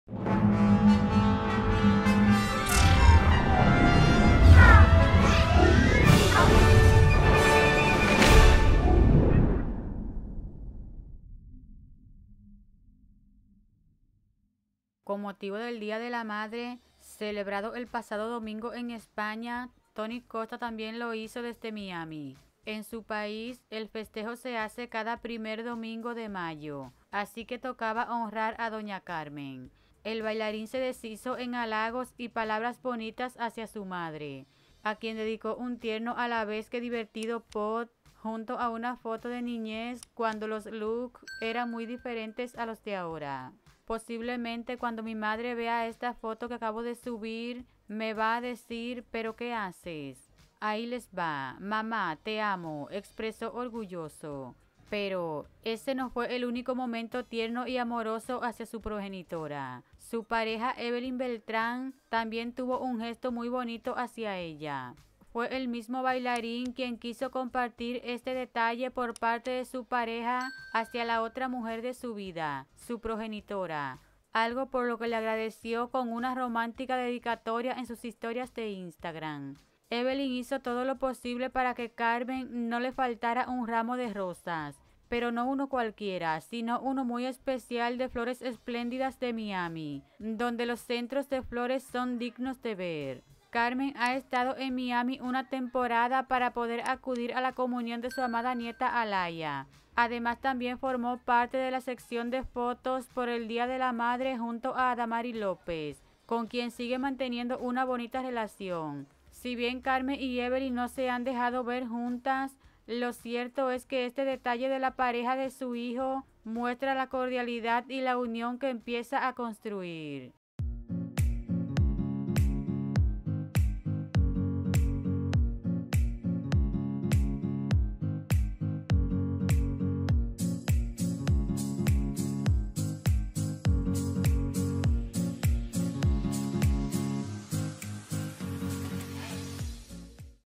Toni Costa PRESUME el bonito GESTO de Evelyn Beltrán con su mamá por el Día de la Madre.
La novia del bailarín tuvo este amoroso detalle con Carmen en un día tan especial.